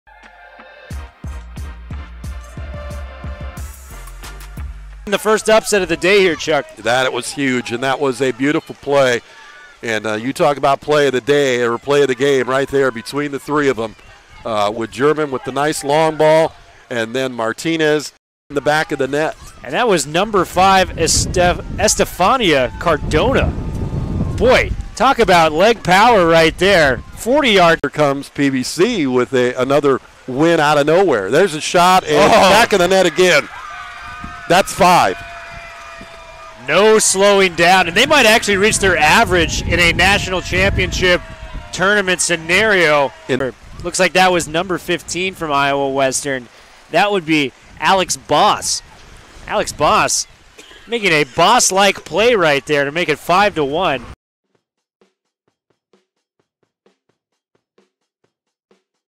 2018 NJCAA DI Women's Soccer Championship - Iowa Western vs. Paradise Valley
No. 3 Iowa Western defeated No. 10 Paradise Valley 5-1 in pool play action at the 2018 NJCAA DI Women's Soccer National Championship in Foley, AL